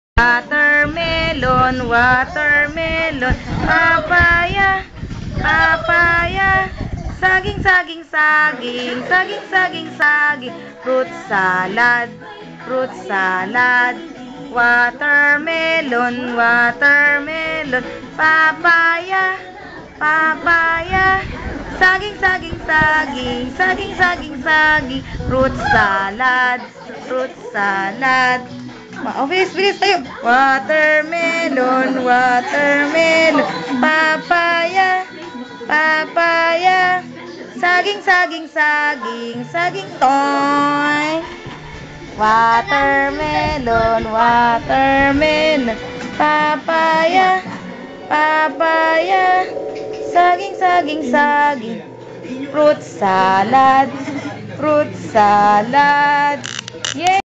ZACKY MIRALLES - WATER MELON
this is the video of 11 month old cute baby boy, Zacky